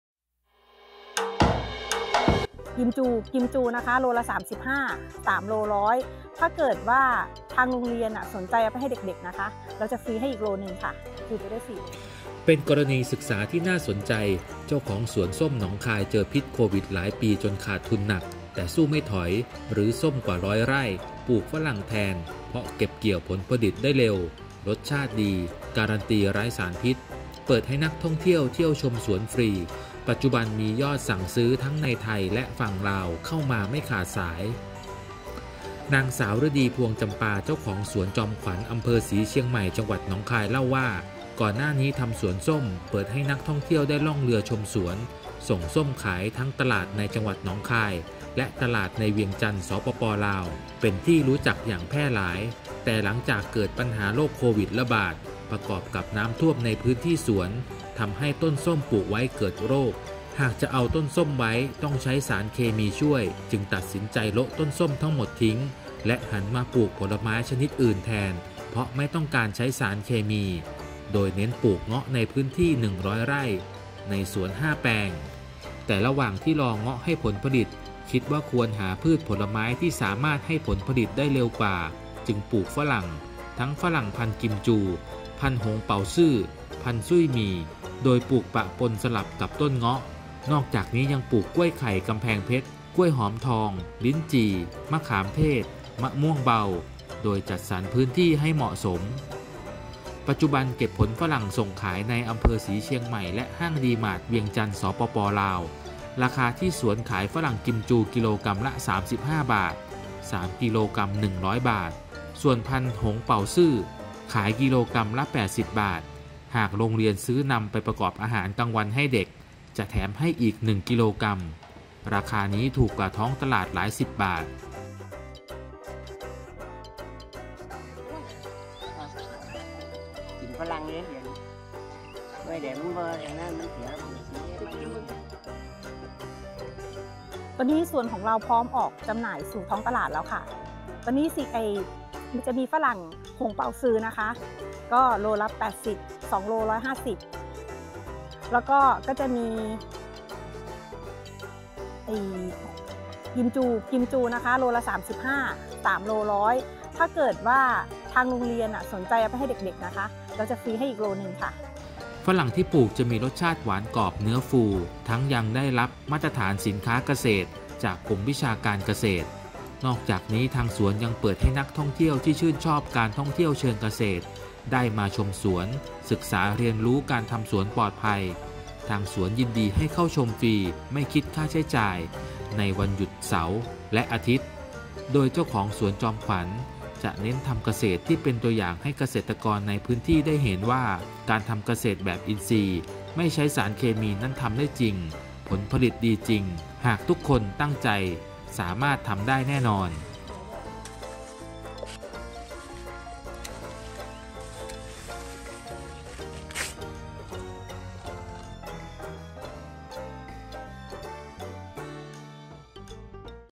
เจอพิษโควิดไม่ถอย! รื้อสวนส้มปลูกฝรั่งอินทรีย์ ขายดีออเดอร์ล้นส่งไม่ทัน : เรื่องเด่นทั่วไทย
กรณีศึกษาที่น่าสนใจ..เจ้าของสวนส้มที่หนองคาย เจอพิษโควิดหลายปีจนขาดทุนหนัก แต่สู้ไม่ถอย รื้อส้มกว่าร้อยไร่ ปลูกฝรั่งแทน เพราะเก็บผลผลิตได้เร็ว รสชาติดี การันตีไร้สารพิษ เปิดให้นักท่องเที่ยว เที่ยวชมสวนฟรี ปัจจุบันมียอดสั่งซื้อไทยทั้งในไทยและฝั่งลาวเข้ามาไม่ขาดสาย